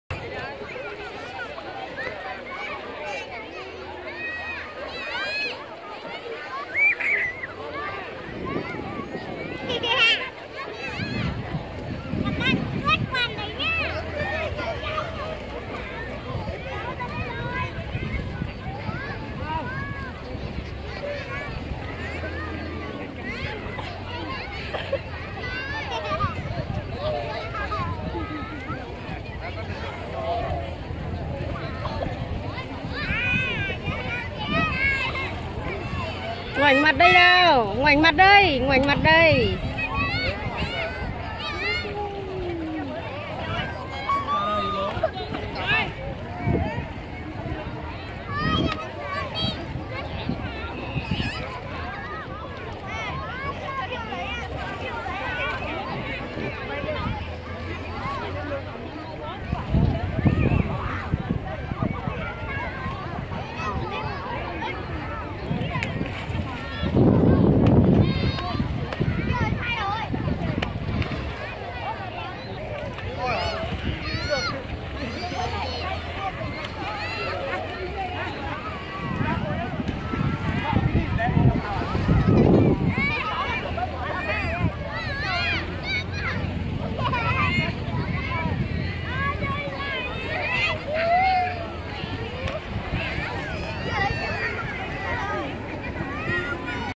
Bé đi du lịch Hồ Đại Lải, Xuân Hòa, Phúc Yên, Vĩnh Phúc
Bé tắm ở hồ Đại Lải, Xuân Hòa, Phúc Yên, Vĩnh Phúc
Du lịch hồ Đại Lải